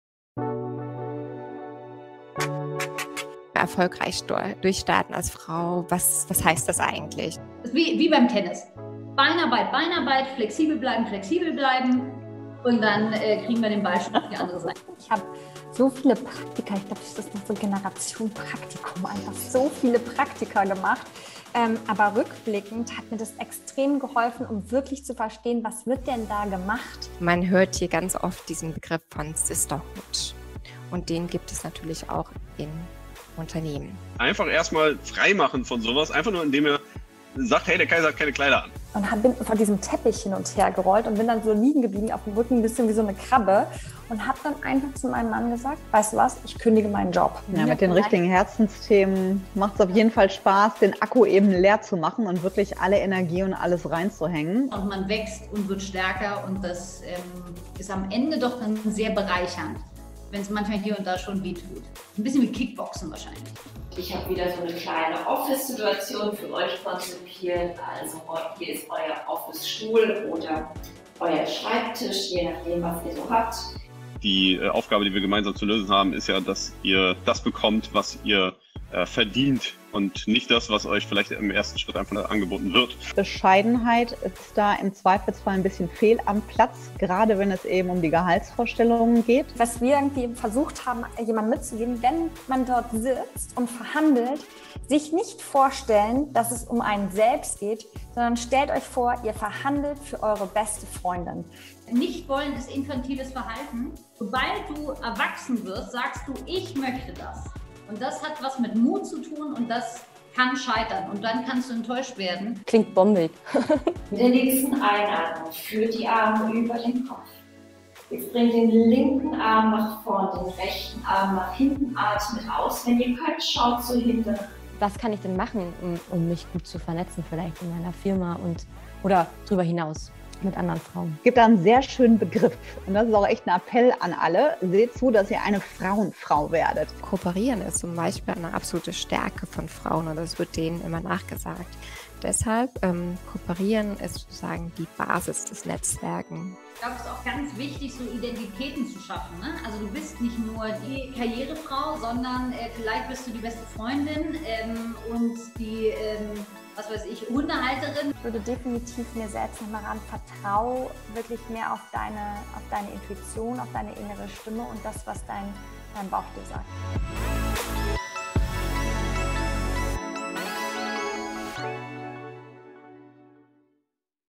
How it works - Highlights: "Die Durchstarterinnen"
„Die Durchstarterinnen: Was du von erfolgreichen Frauen für deinen Weg lernen kannst“

Zum Finale von How it works am 29. Juni 2021 berichteten die Expertinnen Ariane Alter, Moderatorin, Journalistin und Podcasterin, Jenni Baum-Minkus, Gründerin und Geschäftsführerin von gitti, und Nina Michahelles, Industry Lead Consumer Brand bei Google, im Gespräch mit Cathrin Schmiegel von ZEIT CAMPUS über die Anfänge ihrer Karrieren und gaben den TeilnehmerInnen Tipps um richtig im Job durchzustarten.

In diesem Video findest du die Highlights des Thementags. Mehr Informationen rund um die Themen Karriere-Start und Job-Wahl findest du